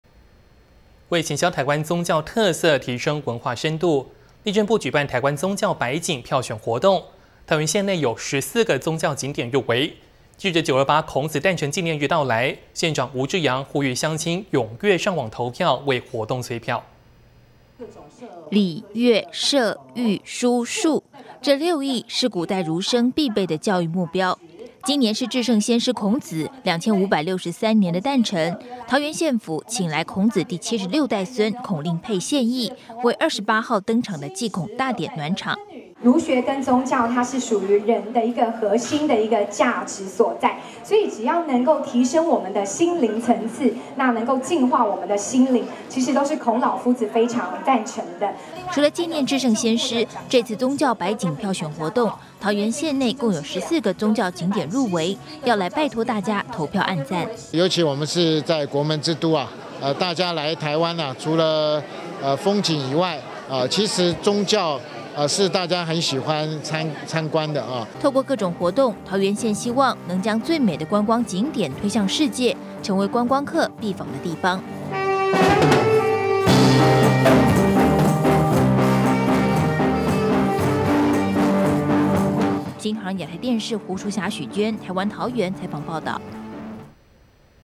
【孔子誕辰_美麗台灣】孔夫子誕辰紀念 為宗教百景催票
【孔子誕辰_美麗台灣】孔夫子誕辰紀念 為宗教百景催票(NTDAPTV) 為行銷台灣宗教特色，提升文化深度，內政部舉辦台灣宗教百景票選活動，桃園縣內有14個宗教景點入圍，藉著928孔夫子誕辰紀念日的到來，縣長吳志揚呼籲鄉親踴躍上網投票，為活動催票。【新唐人亞太台2013年9月20日訊】

禮、樂、射、御、書、數，這六藝是古代儒生必備的教育目標；今年是至聖先師孔子2563年的誕辰，桃園縣府請來孔子第七十六代孫孔令珮獻藝，為28號登場的祭孔大典暖場。

孔子第七十六代子孫 孔令珮：「儒學跟宗教它是屬於人的一個核心價值所在，所以只要提升我們人的心靈層次，能淨化我們的心靈，其實都是孔老夫子非常贊成的。」

桃園縣長 吳志揚：「尤其我們是在國門之都，大家來台灣，除了風景以外，其實宗教是大家很喜歡參觀的。」

透過各種活動，桃園縣希望能夠將最美的觀光景點推向世界，成為觀光客必訪的地方。